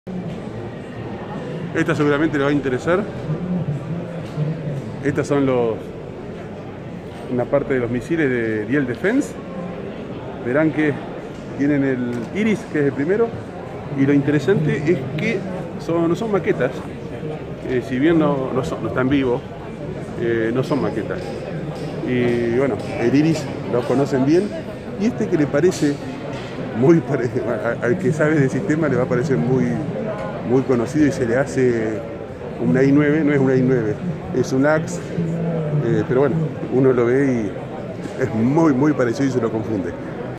FIDAE 2022: STAND DE DIEHL DEFENCE
Diariamente tratare de dejarle videos de los STAND mas reconocidos de la FIDAE 2022.
En este caso es de la empresa DIEHL DEFENCE es un fabricante alemán de armas y una división de la Diehl Stiftung con sede en Überlingen, produce principalmente misiles y municiones.

En 1960, Bodenseewerke Gerätetechnik se convirtió en el contratista principal para la producción europea del misil aire-aire estadounidense Sidewinder AIM-9B. Basado en la experiencia con el Sidewinder, Diehl desarrolló un nuevo buscador progresivo para un nuevo tipo de misil aire-aire de corto alcance: el IRIS-T.

En el STAND se pudieron verl el misil IRIS-T que es un misil aire-aire infrarrojo de corto alcance para reemplazar el AIM-9 Sidewinder que se encuentra en algunos países miembros de la OTAN. Cualquier avión capaz de disparar el Sidewinder también es capaz de lanzar el IRIS-T.

El otro misil era el Laser-Guided Sidewinder (LaGS) es una versión aire-superficie desarrollada em base al misil aire-aire AIM-9L Sidewinder pensado para atacar objetivos móviles pequeños y blindados ligeros en tierra. Utiliza una ojiva pequeña con la máxima precisión de objetivo para minimizar los daños colaterales, el buscador de infrarrojos del AIM-9L ha sido reemplazado por un láser semiactivo (SAL) en el misil LaGS.
Lo mas interesante es que es totalmente compatible con las estaciones e interfaces de misiles aire-aire Sidewinder existentes. El ciclo de vida de la nueva arma es de 20 años.

Próximamente saldra un video sobre este ultimo sistema.

Si queres podes invitarme un CAFE por esto medios!!
Café que por supuesto agradeceré en los vivos u otros videos!!